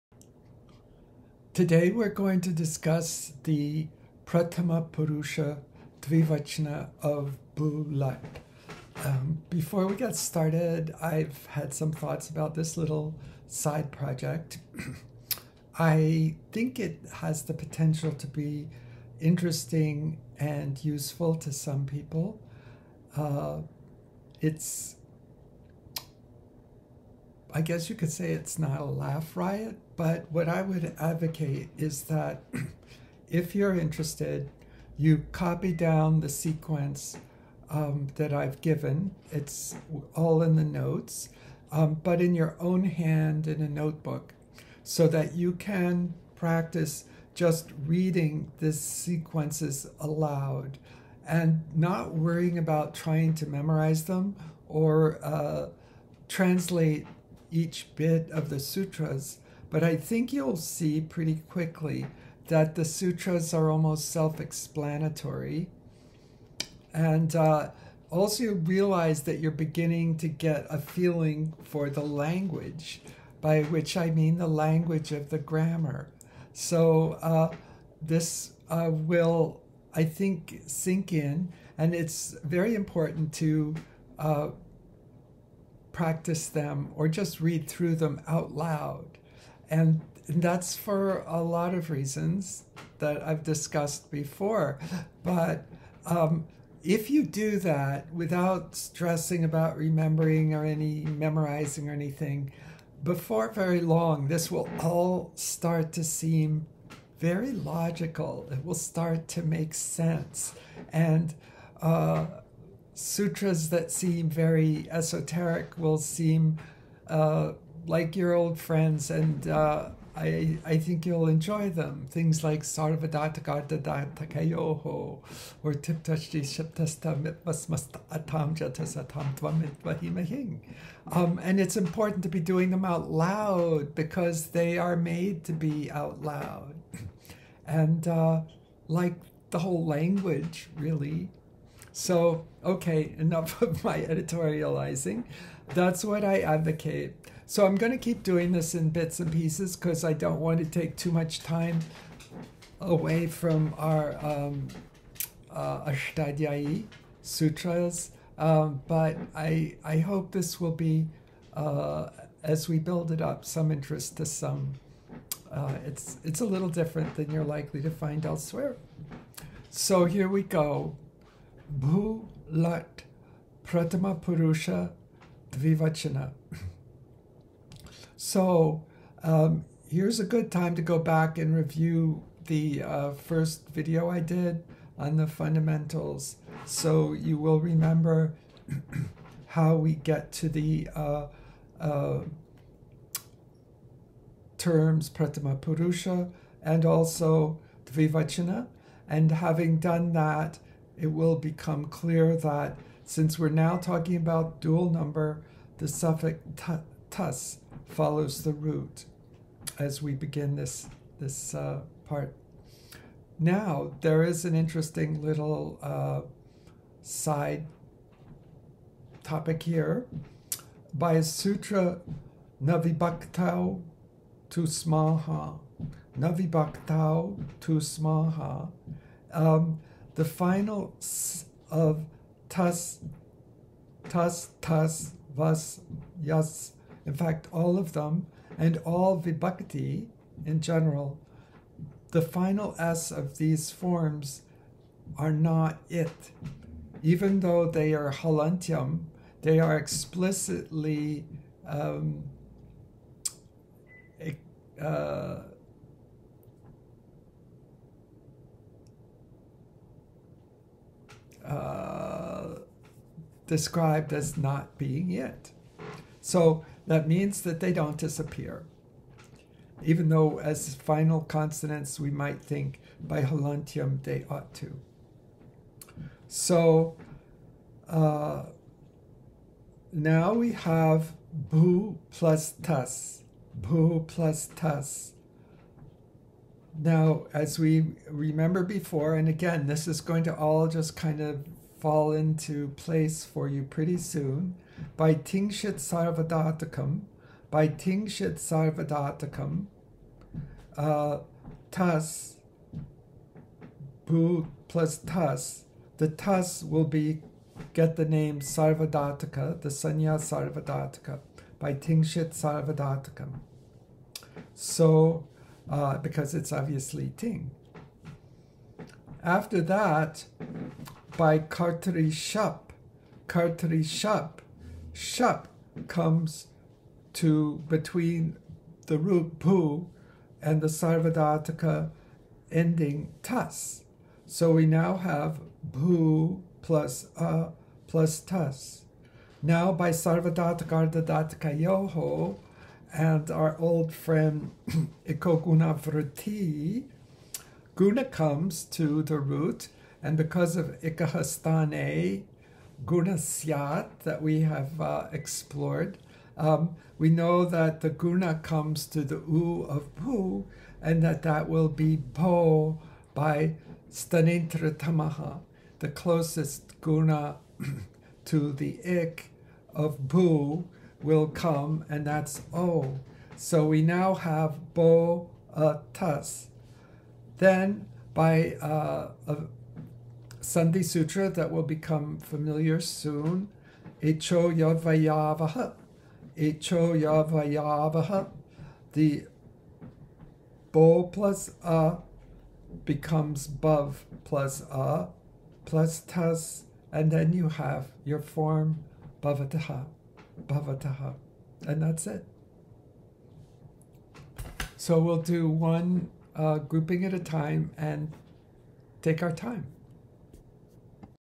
भू लट् (3) प्रथमपुरुष द्विवचन
Since the agent is now of dual number, the suffix तस् follows the root.
Note - by न विभक्तौ तुस्मा: १.३.४ (190) the final स् of तस् थस् वस् यस् etc. (or of any विभक्ति ) does not get इत् संज्ञा by हलन्त्यम् १.३.३ (1).

भू + तस्
तस् gets सार्वधातुकम् संज्ञा by तिङ् शित् सार्वधातुकम् ३.४.११३ (2166)
भू + शप् + तस् by कर्तरिशप् ३.१.६८ (2167)
भू + अ + तस्   by  लशक्वतध्दिते १.३.८ (195) and  हलन्त्यम् १.३.३ (1)
भो + अ + तस्  by  सार्वधातुकार्धधातुकयोः ७.३.८४ (2168)
भव् + अ + तस्  by एचोऽयवायावः ६.१.७८ (61)
भवतः